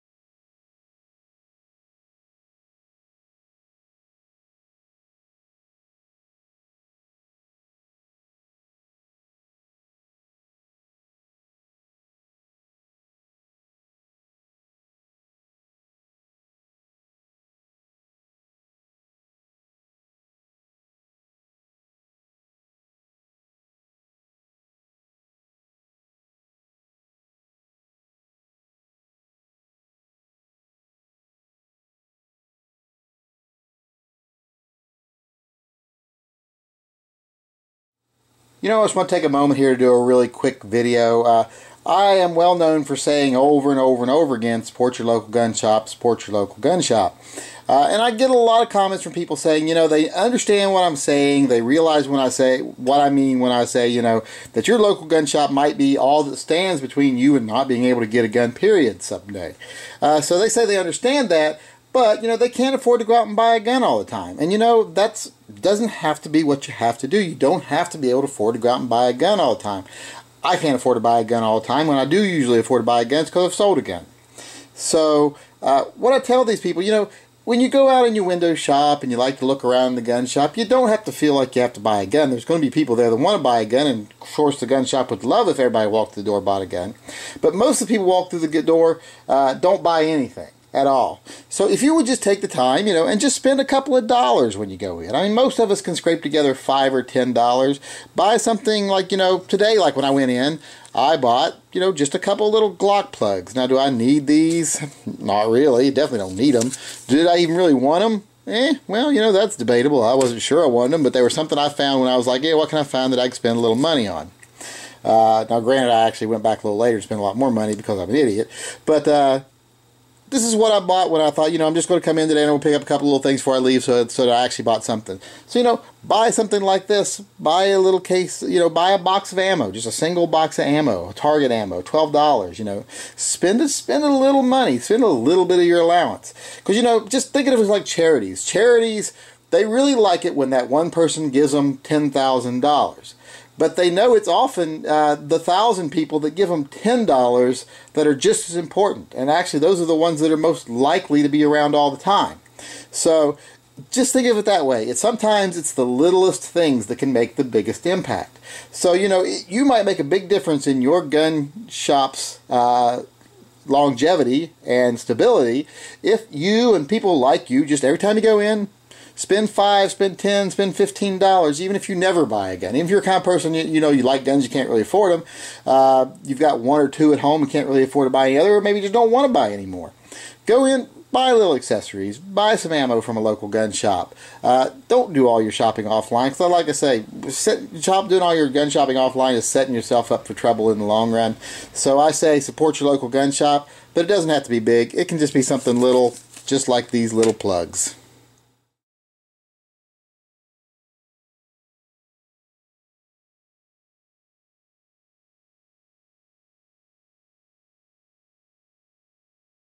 Black Friday: Show Your LGS Some Love
Remember to shop locally if at all possible this Black Friday. Your local gun shops could use the love.